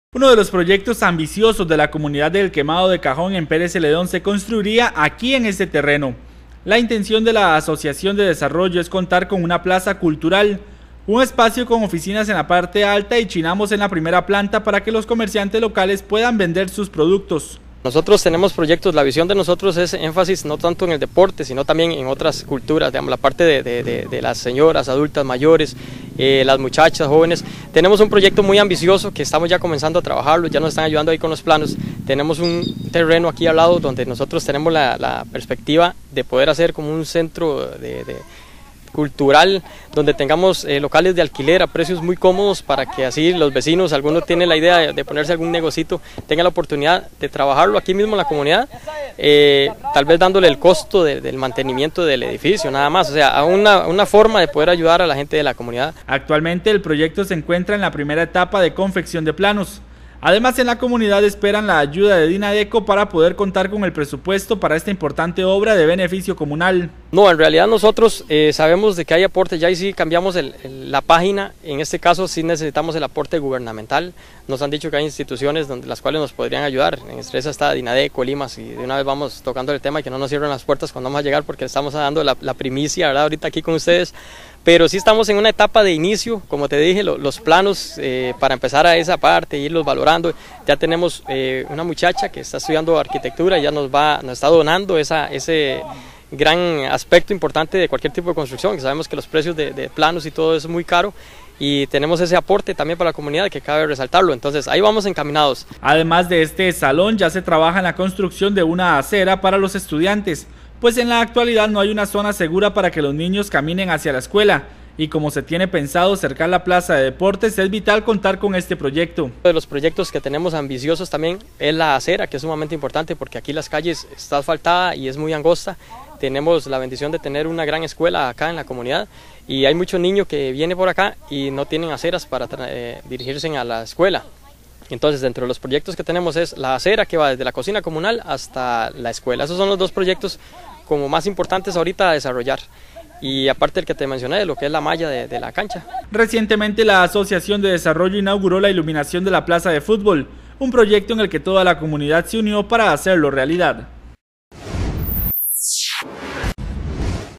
En El Quemado de Cajón esperan construir una plaza cultural para los comerciantes
Uno de los proyectos ambiciosos de la comunidad de El Quemado de Cajón en Pérez Zeledón se construiría aquí en este terreno, la intención de la Asociación de Desarrollo es contar con una plaza cultural, un espacio con oficinas en la parte alta y chinamos en la primera planta para que los comerciantes locales puedan vender sus productos.